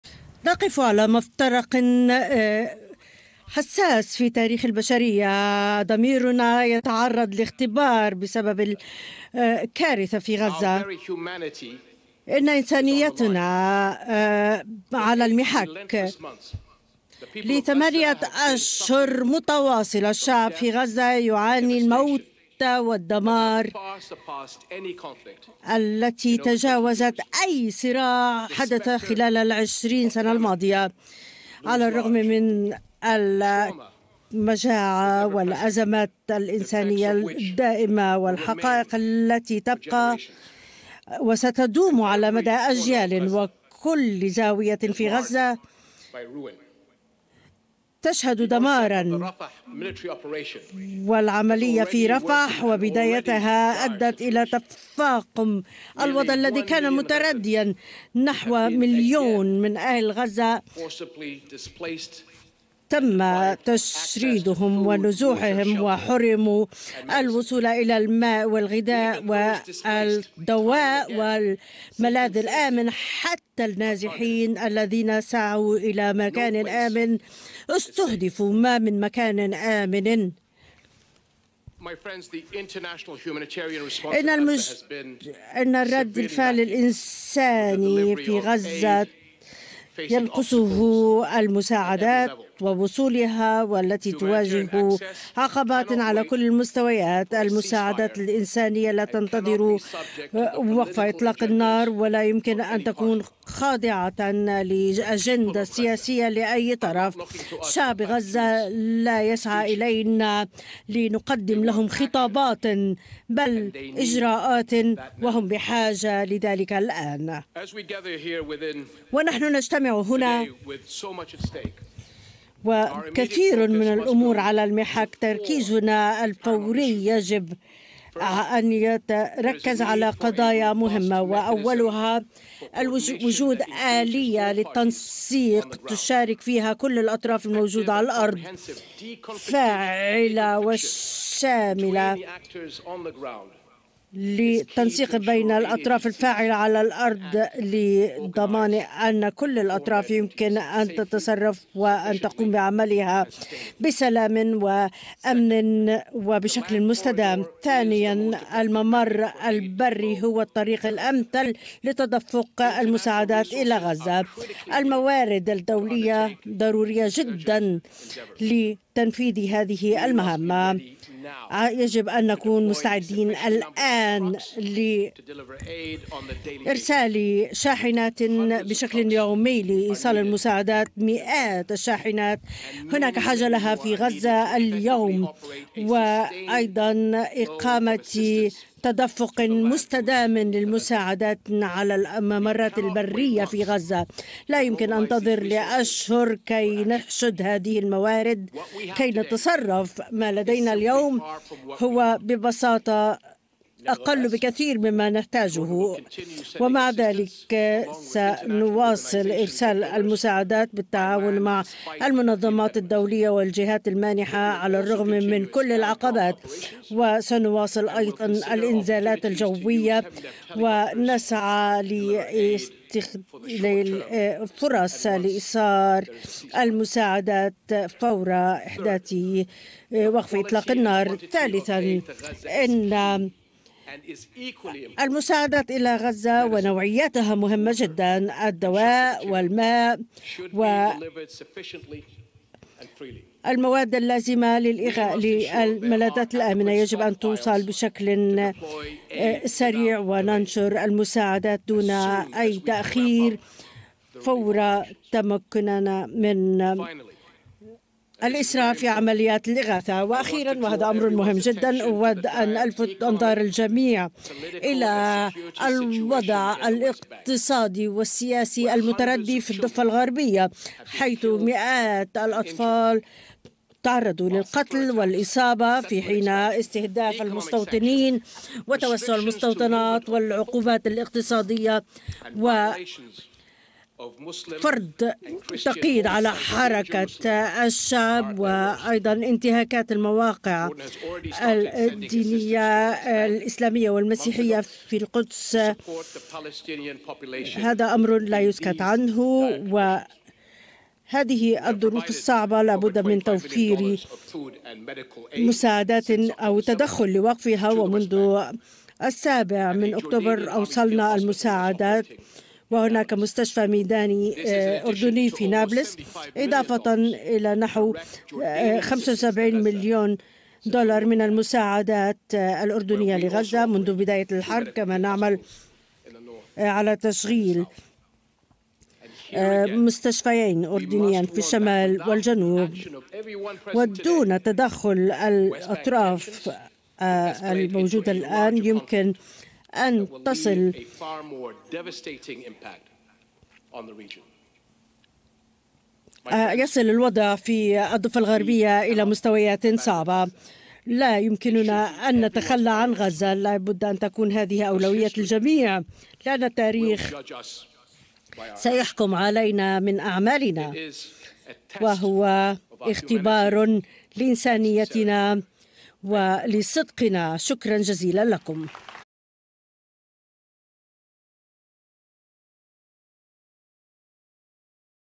كلمة ملك الأردن خلال مؤتمر الاستجابة الإنسانية الطارئة في غزة
كلمة العاهل الأردني الملك عبدالله_الثاني خلال مؤتمر الاستجابة الإنسانية الطارئة في